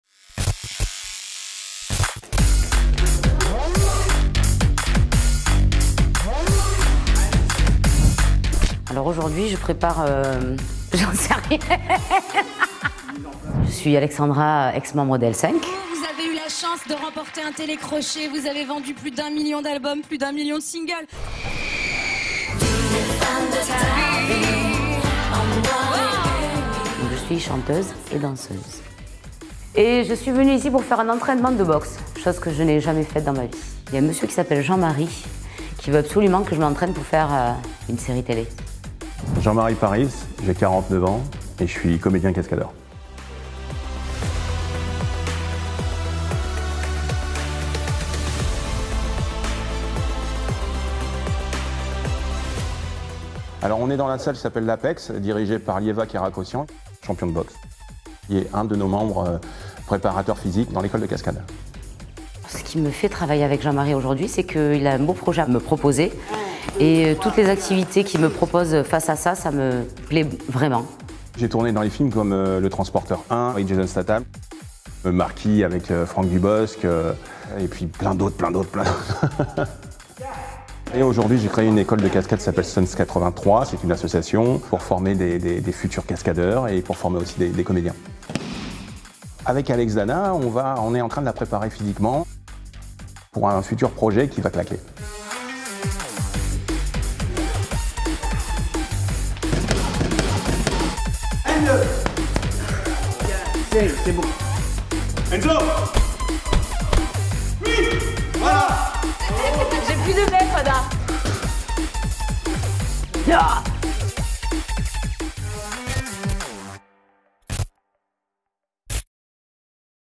Tu veux me voir mettre un KO sur un ring de boxe ? 🥊
Un domaine que je n'aurais jamais imaginé toucher de si près: la boxe.

J'ai eu le plaisir de prendre un cours auprès du champion Lyova Kirakosyan dans sa salle "l'Apex" à Toulon.

Pour tout vous dire je me suis éclatée!!!
Un grand merci à Paris Jean Marie, Stunts83,  de me proposer autant de nouvelles activités autour du but que tu t'es fixé et pour lequel je me battrais!

Merci à Madeinvar.tv pour ce reportage et ce montage qui me ressemble plutôt bien.
La suite au prochain épisode!